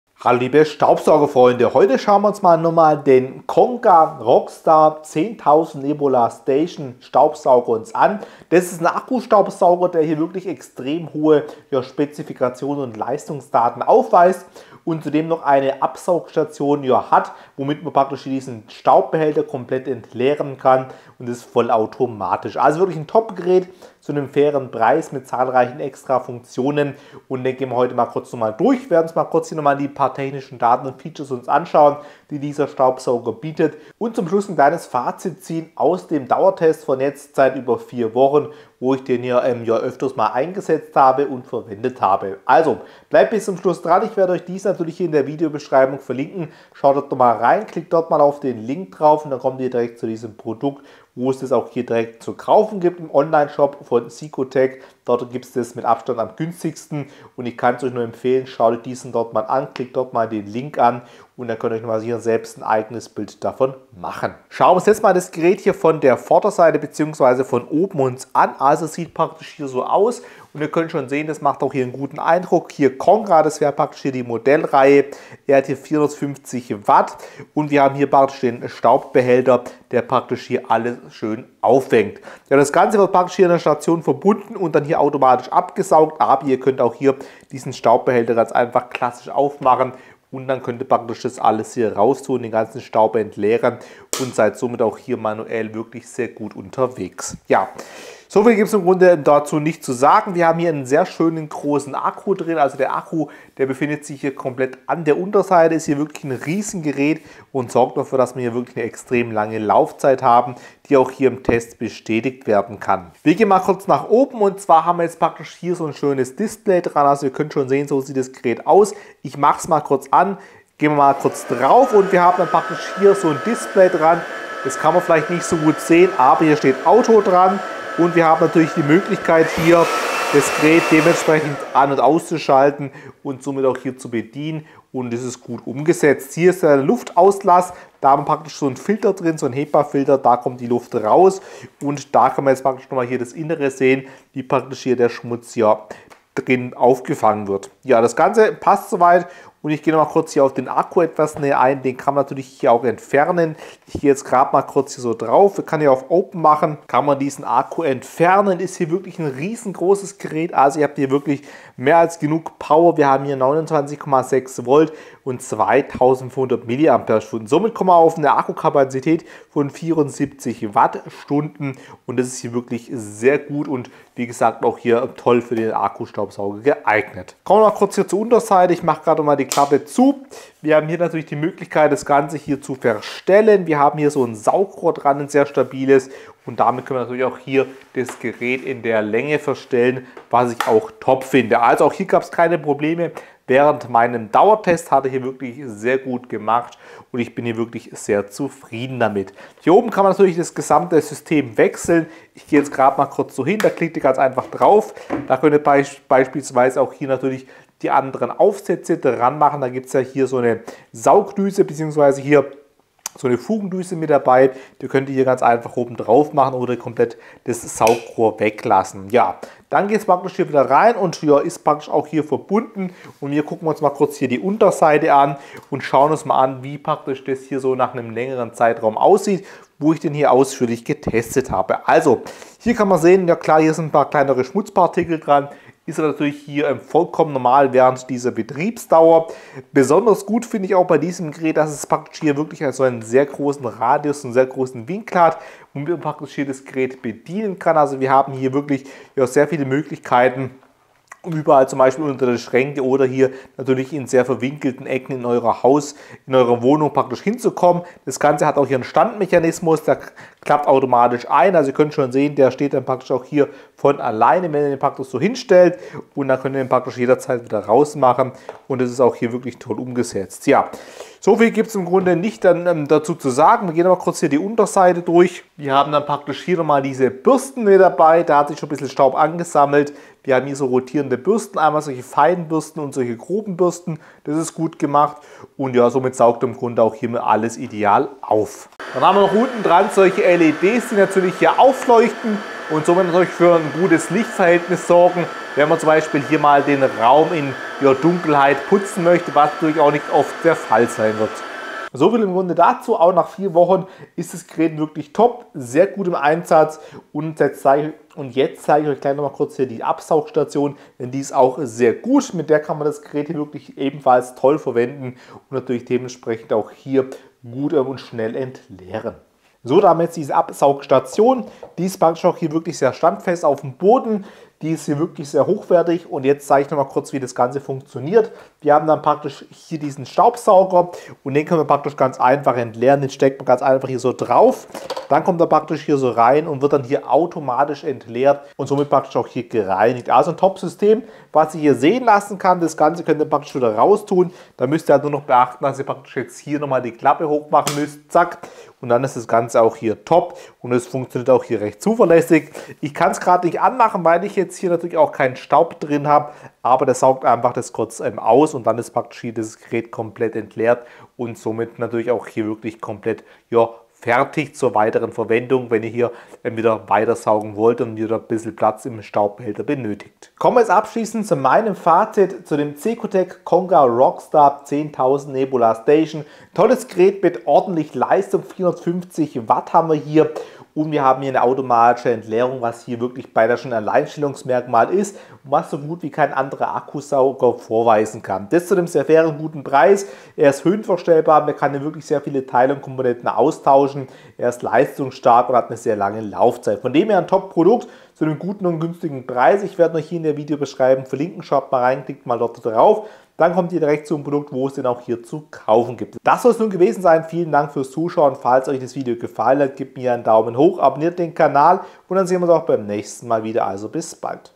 Cecotec Akku-Staubsauger Test || Conga Rockstar 10000 Nebula Station
Cecotec Conga Rockstar 10000 Nebula Station
Test (Praxistest) des Akkusaugers

Hallo Zusammen,
in diesem Video wollen wir von euch die beliebte Cecotec Conga Rockstar 10000 Nebula Station im Test ganz genau vorstellen.

Diesmal liegt der Fokus auf dem herausragenden Cecotec Conga Rockstar 10000 Nebula Station. Tauchen Sie ein, während wir alle Vorteile dieses beeindruckenden Staubsaugers herausstellen und die technischen Daten im Detail erläutern.

Akku Staubsauger Test Highlights: In diesem Akku Staubsauger Test stehen die Leistung und Vielseitigkeit der Cecotec Conga Rockstar 10000 Nebula Station im Mittelpunkt. Wir führen Sie durch jeden Schritt des Tests und zeigen Ihnen, wie dieser Staubsauger auf unterschiedlichen Oberflächen wie Fliesenböden und Teppichen glänzt.

Technische Daten im Überblick: Mit einer maximalen Leistung von 450 W und einer beeindruckenden Saugkraft von 30 kPa bietet die Cecotec Conga Rockstar 10000 Nebula Station eine erstklassige Reinigungsleistung. Der Lithium-Ionen-Akku mit 2200 mAh und 33,6 V ermöglicht eine Autonomie von bis zu 65 Minuten, sodass Sie Ihr gesamtes Zuhause ohne Unterbrechungen reinigen können.

Die Vorteile im Test: Wir heben die zahlreichen Vorteile hervor, die sich aus den verschiedenen Tests ergeben haben. Von der einfachen Handhabung über die effektive Reinigung bis hin zur praktischen Absaugstation - die Cecotec Conga Rockstar 10000 Nebula Station bietet alles, was Sie sich von einem hochwertigen Akku Staubsauger wünschen.

Fazit des Tests: Nachdem wir die Cecotec Conga Rockstar 10000 Nebula Station ausführlich getestet haben, können wir mit Zuversicht sagen, dass sie unsere Erwartungen übertroffen hat. Mit ihrem kraftvollen Motor, der langen Akkulaufzeit und den vielseitigen Reinigungsoptionen ist dieser Staubsauger eine klare Empfehlung für alle, die nach einer effizienten und zuverlässigen Reinigungslösung suchen.

* Afilliate-Links

Vielen Dank also für deine Unterstützung !